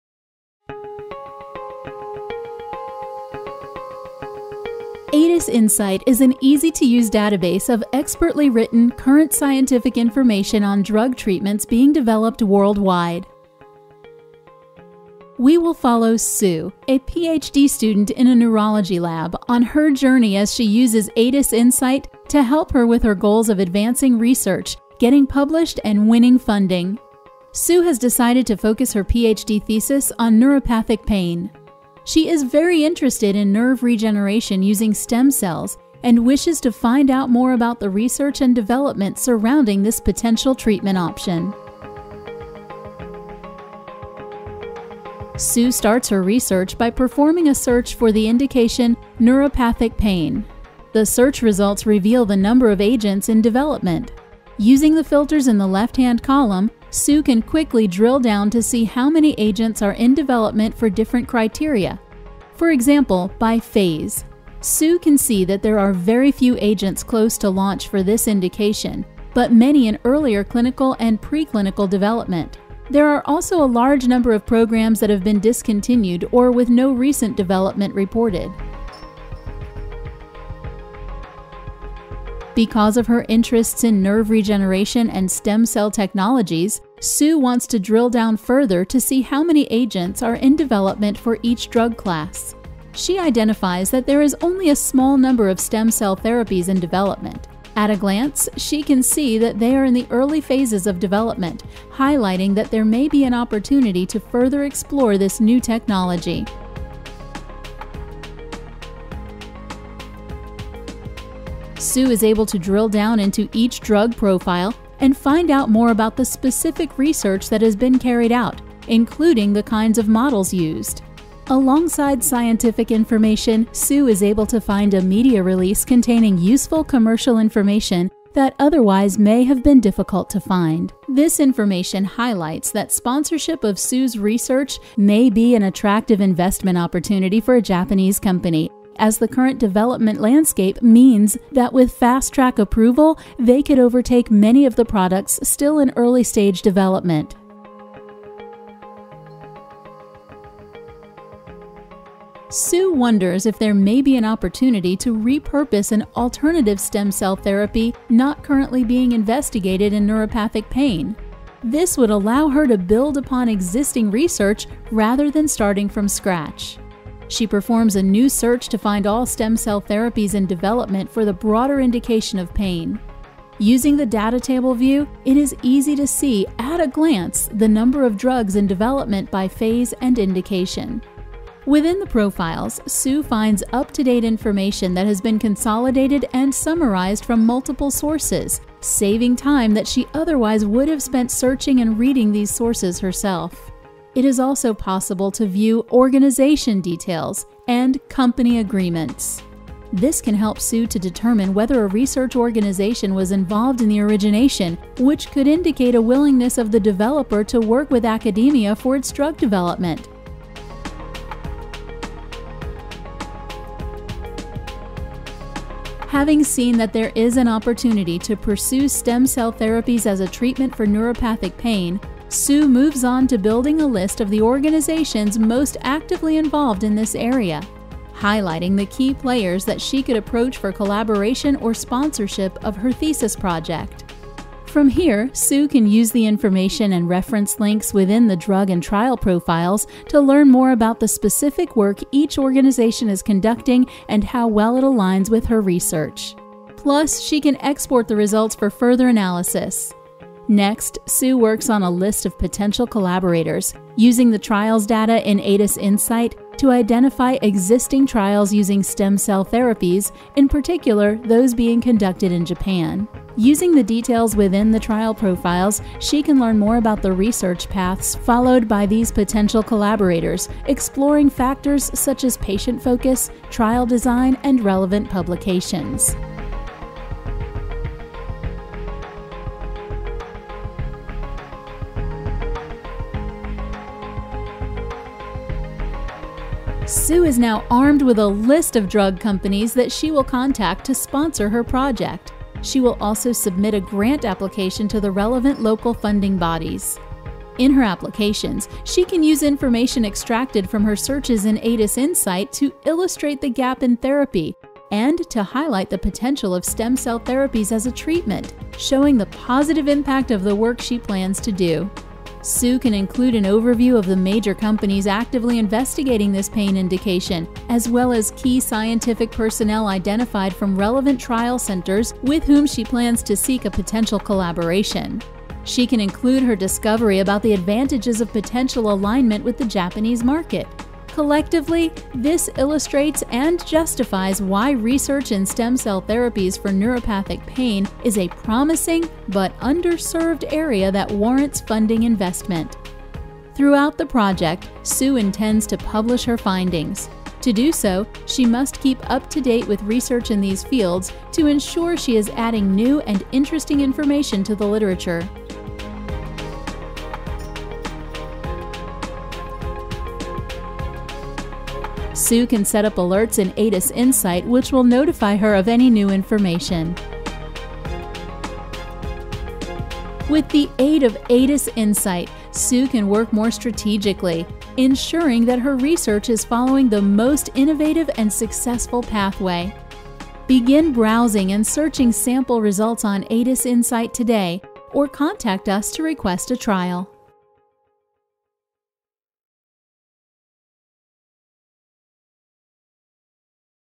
Using AdisInsight to support academic drug research
Follow the story of a PhD student and see how the AdisInsight platform can help you to work more strategically to advance research, get published and win funding.

AdisInsight is an easy-to-use database containing expertly-written scientific and commercial information on drugs, clinical trials, safety reports, and company deals.